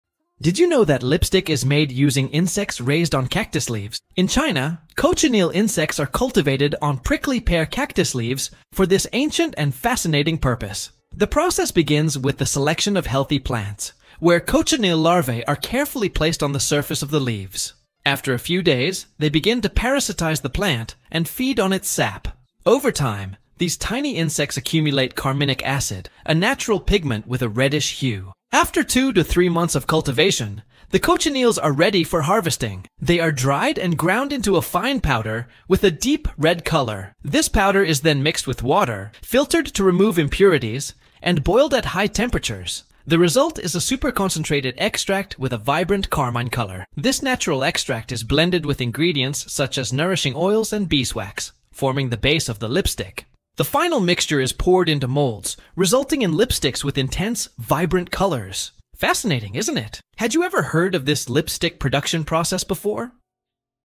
See how cochineal lipsticks are made!
The production of lipsticks using cochineal involves a natural dye called carminic acid, which is extracted from the cochineal insect that lives on cacti. These insects are collected, dried, and crushed to obtain the deep red pigment known as carmine. This dye is widely used in the cosmetics industry to give lipsticks vibrant red and pink shades. It’s a natural and long-lasting colorant, though its use raises ethical concerns due to the involvement of animals.